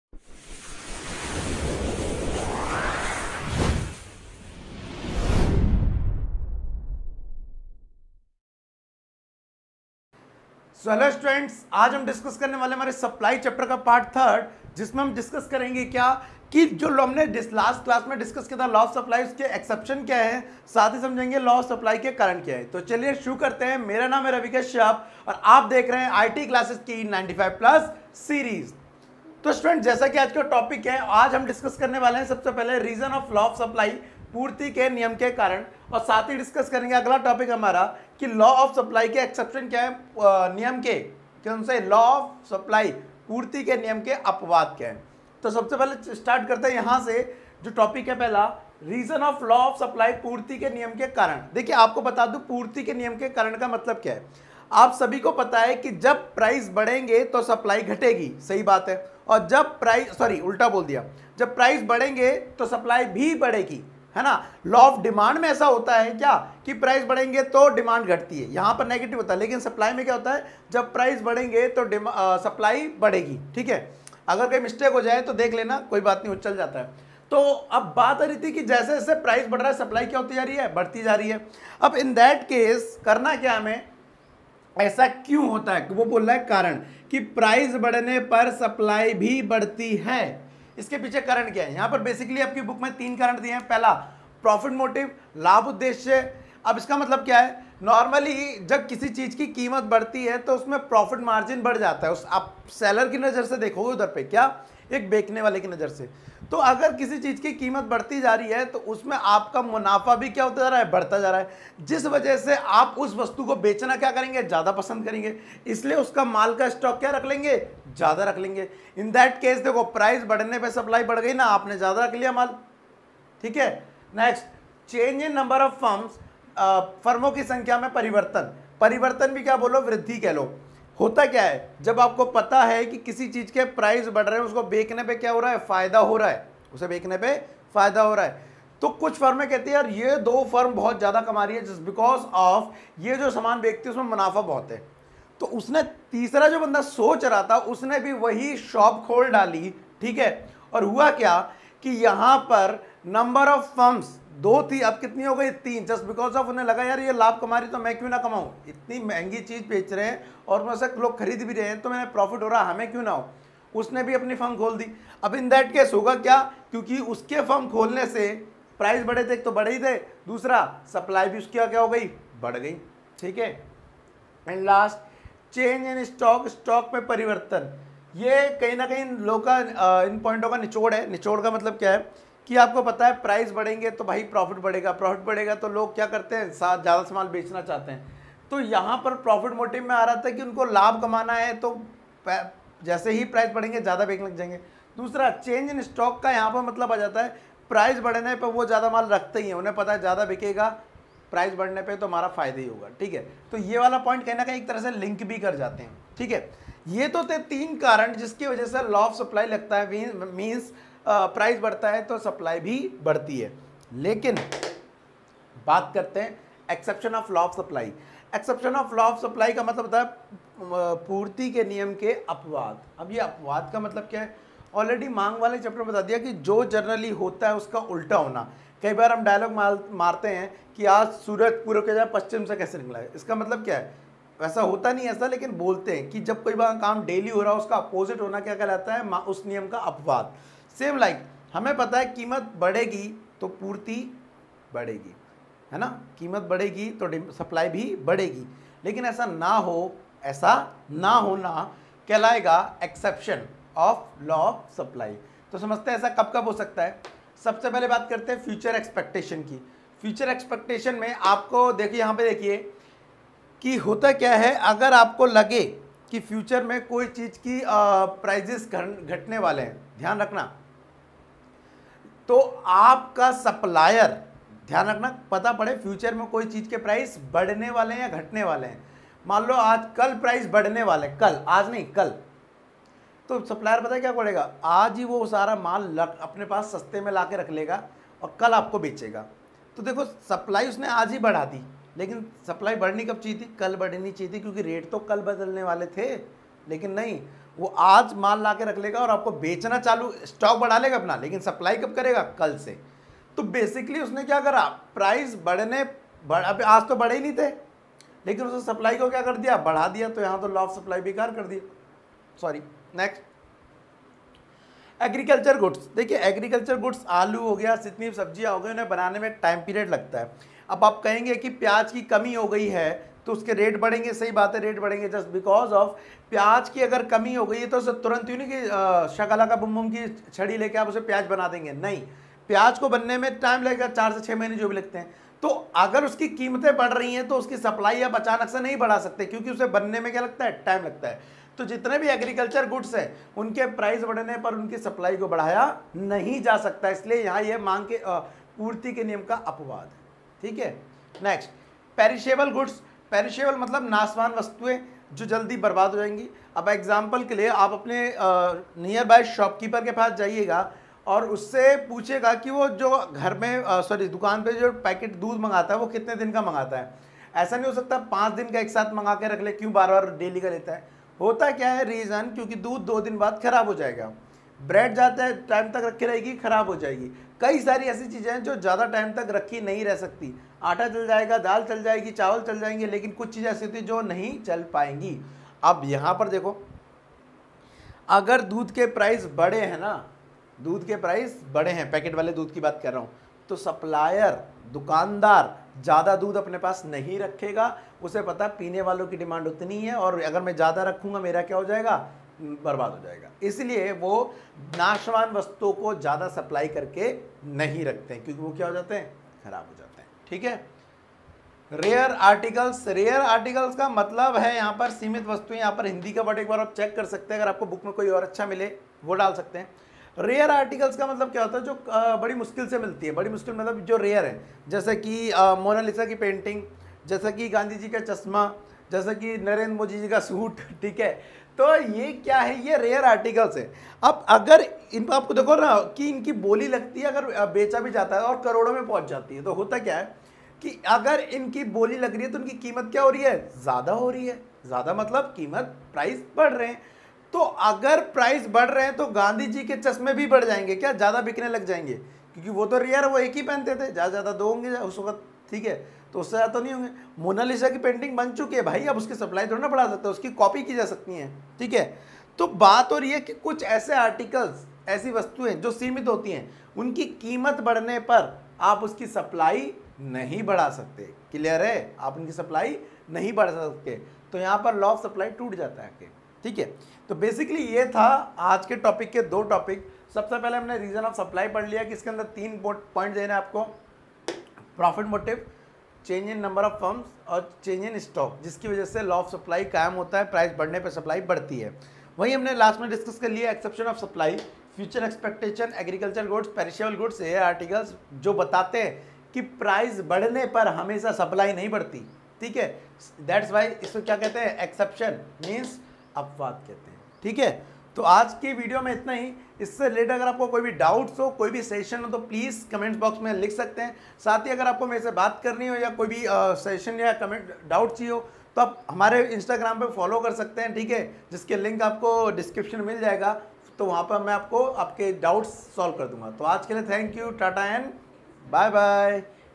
Micro Economics: Supply (Part-3), Chapter-11
Supply is a fundamental economic concept that describes the total amount of a specific good or service that is available to consumers. Supply can relate to the amount available at a specific price or the amount available across a range of prices if displayed on a graph.